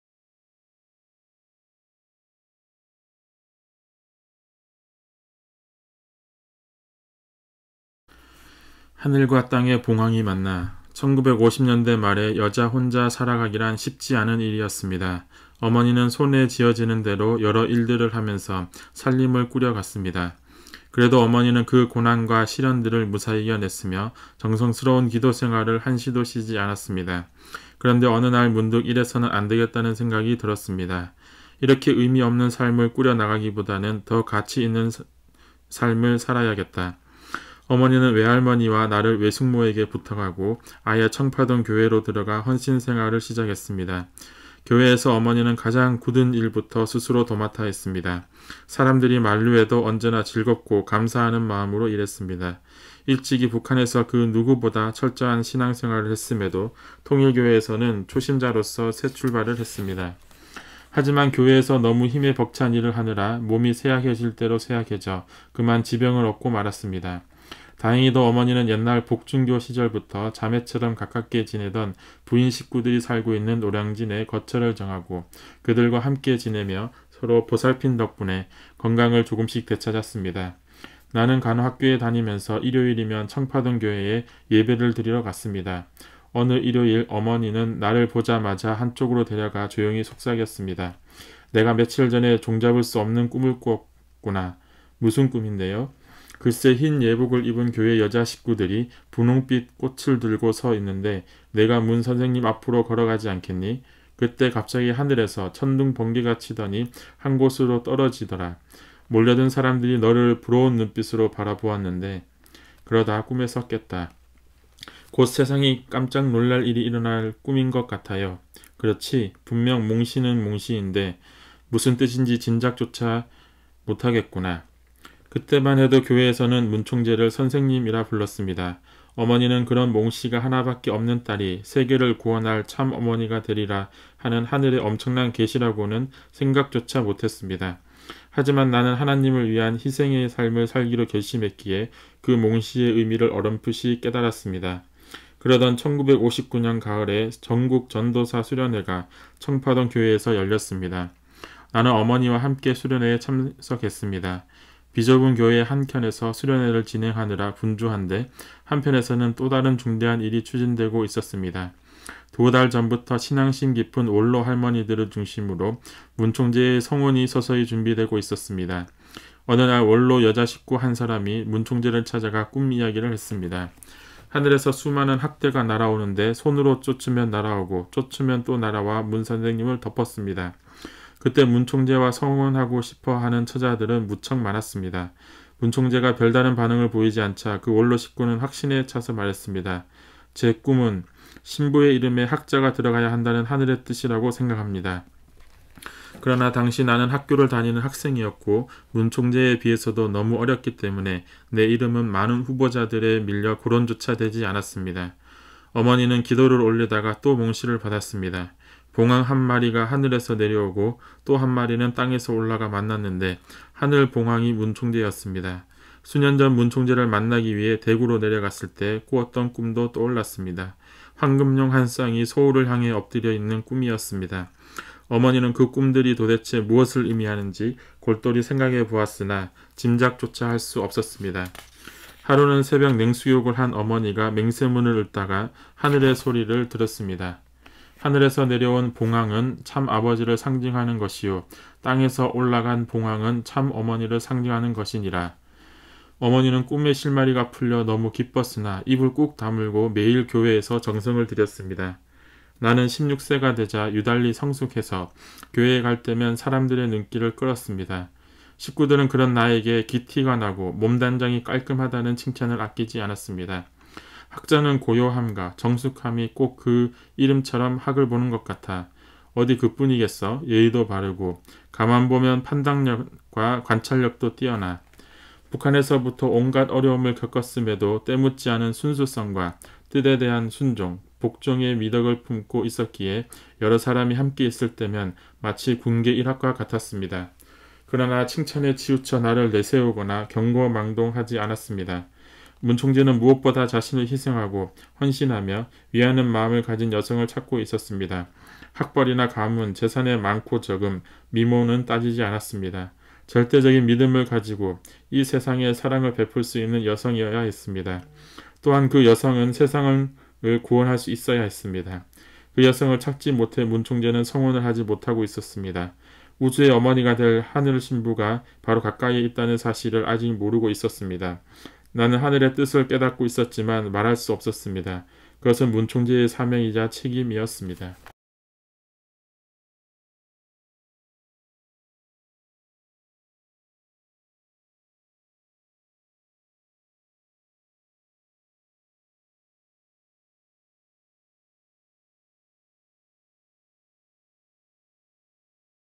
3-3 자서전 "평화의 어머니" - 3장 '어린양 혼인잔치'는 세계를 구원하는 등불 3절 하늘과 땅의 봉황이 만나
하나님의 창조는 남자와 여자! 독생자가 있다면 독생녀를 만나서 결혼하여 참부부, 참부모가 되는 것이 하늘의 섭리가 아닐까요? 그것이 어린 양의 혼인 잔치라는 것을 하루 빨리 깨달을 수 있는 우리 인류가 되기를 기원해 봅니다.

"하나님이 자기 형상 곧 하나님의 형상대로 사람을 창조하시되 남자와 여자를 창조하시고"
구약 창세기 1장 27절
"하나님이 세상을 이처럼 사랑하사 독생자를 주셨으니 이는 그를 믿는 자마다 멸망하지 않고 영생을 얻게 하려 하심이라"
신약 요한복음 3장 16절
"천사가 내게 말하기를 기록하라 어린 양의 혼인 잔치에 청함을 받은 자들은 복이 있도다 하고 또 내게 말하되 이것은 하나님의 참되신 말씀이라 하기로"
신약 요한계시록 19:6